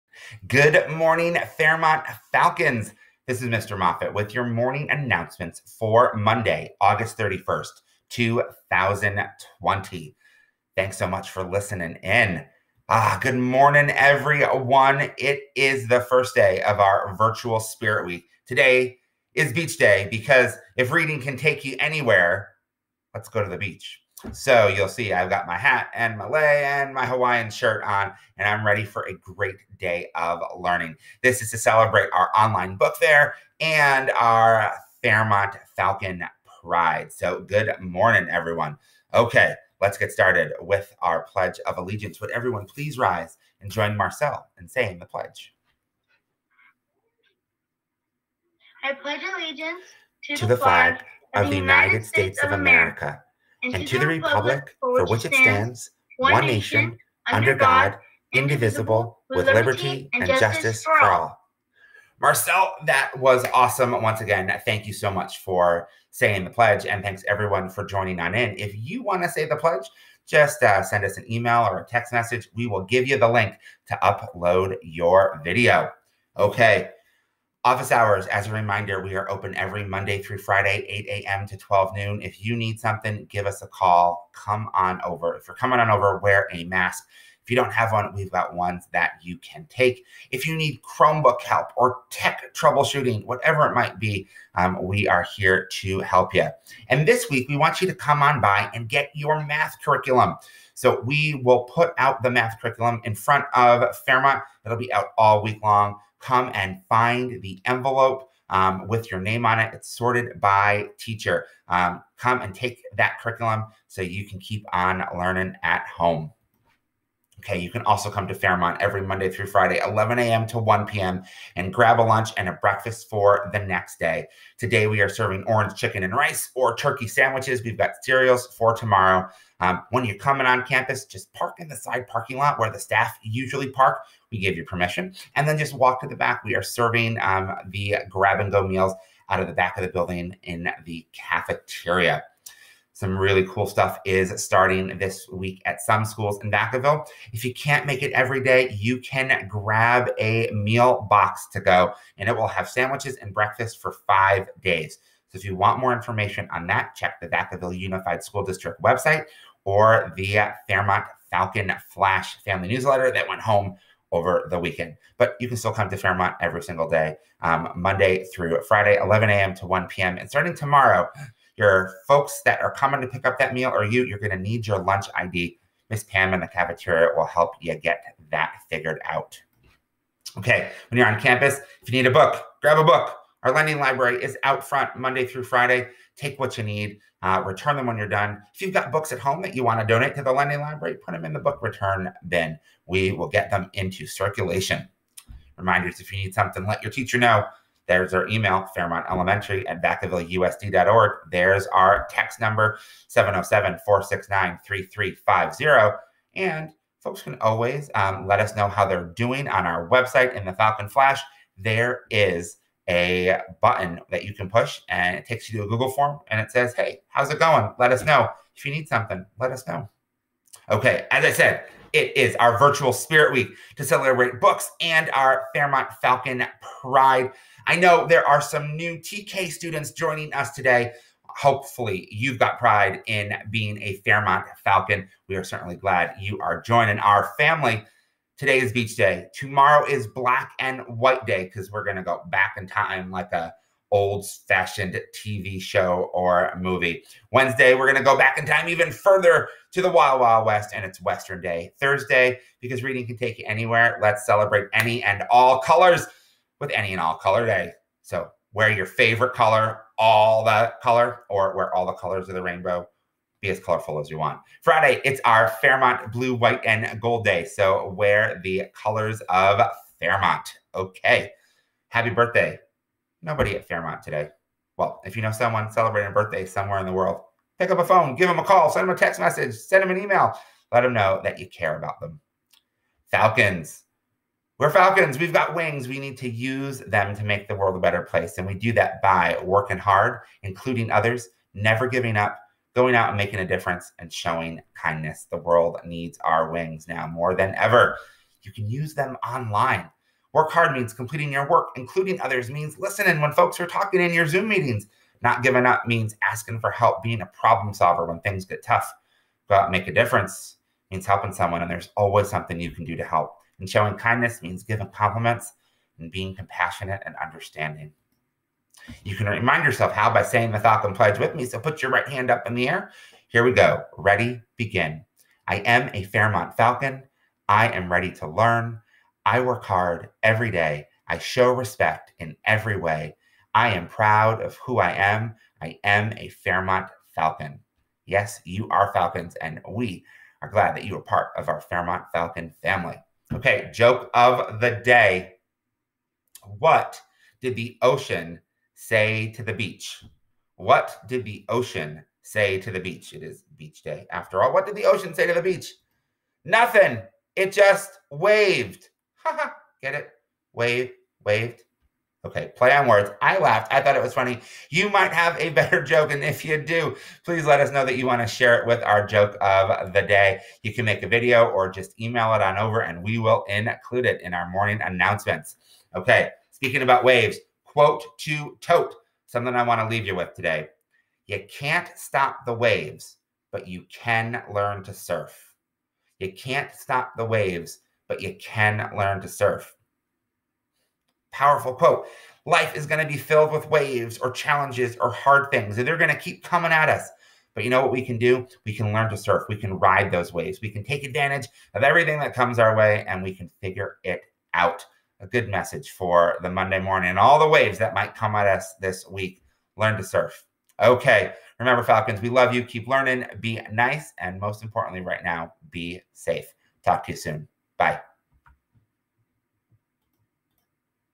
Morning Announcements - August 31, 2020
It's Beach Day and we're ready to ride whatever waves come our way! Here are the Morning Announcements for August, 31, 2020!